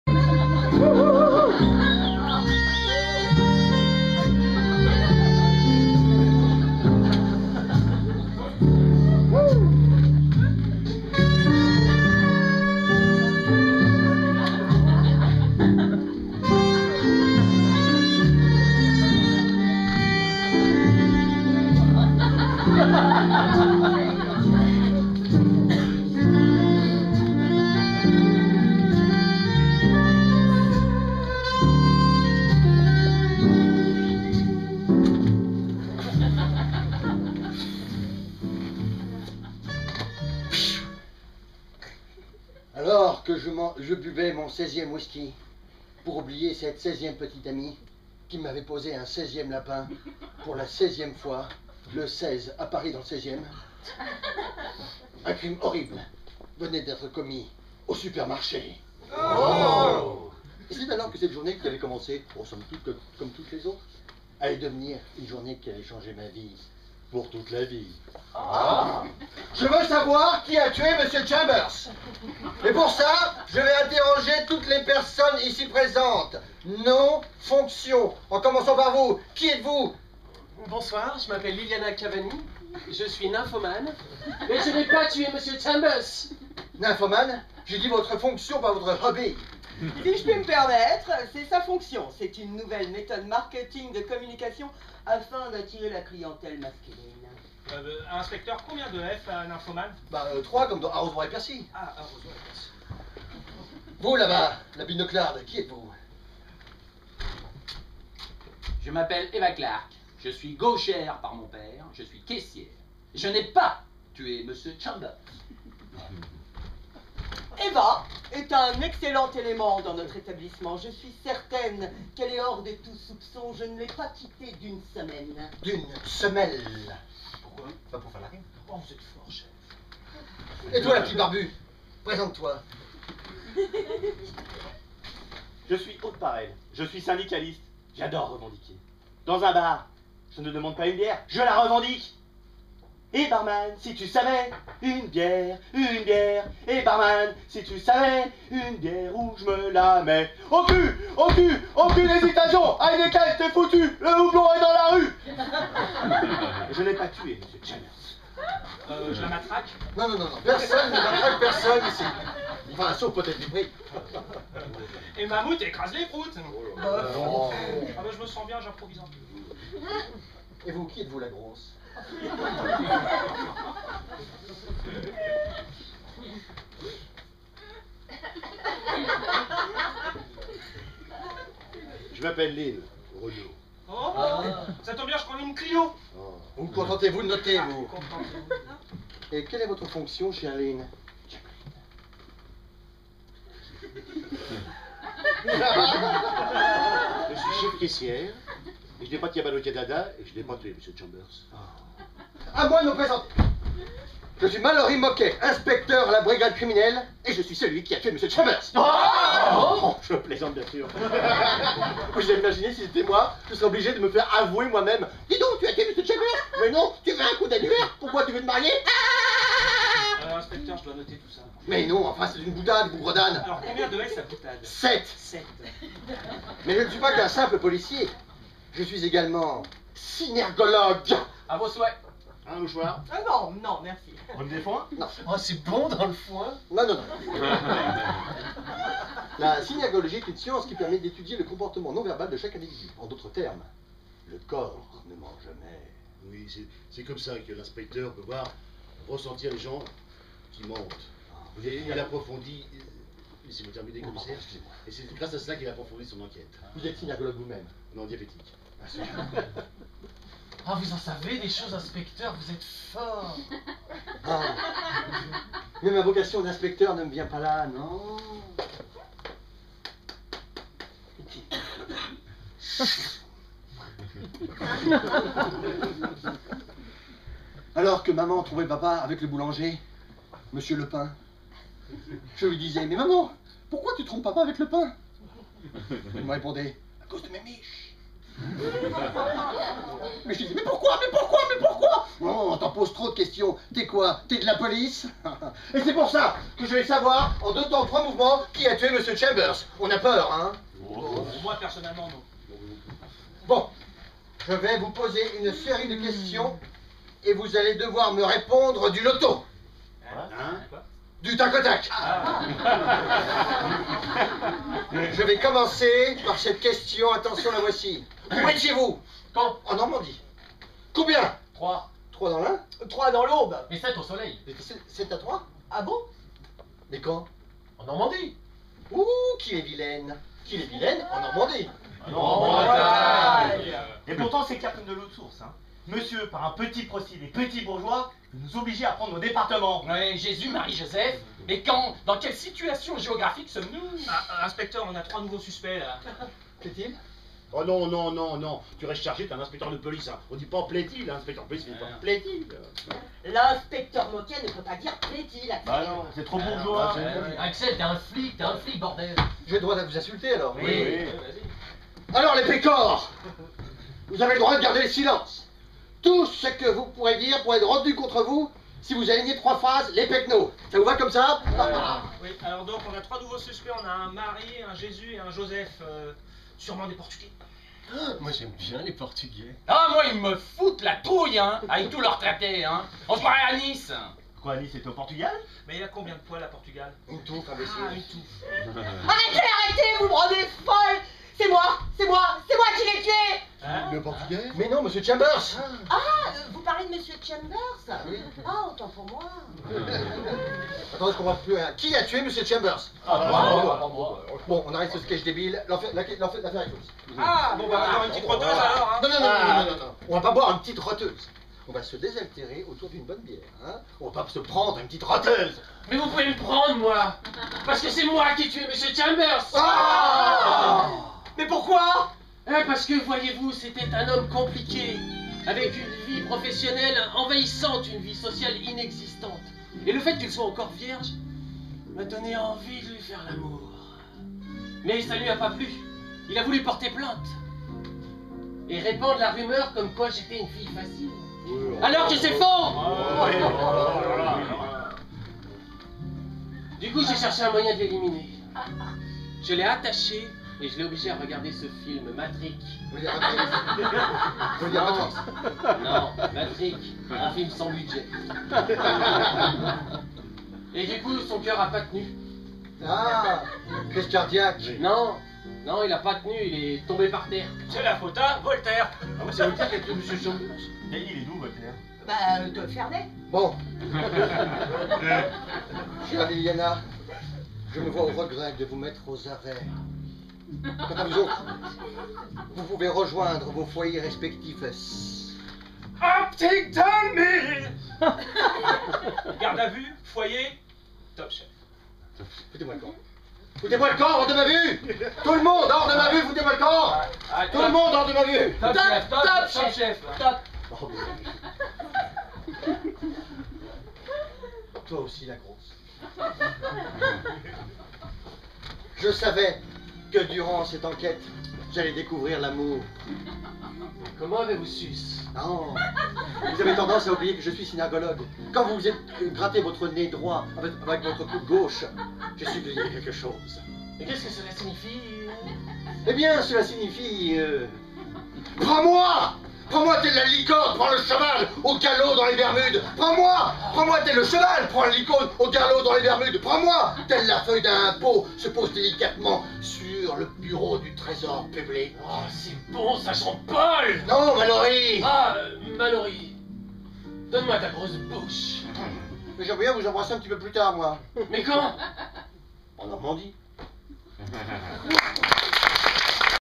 L'Enquête
Un detective mène une enquête sur un meurtre sordide dans un supermarché.

Spectacle du Comedy Club des Têtes de l'Art 74, Juin 2015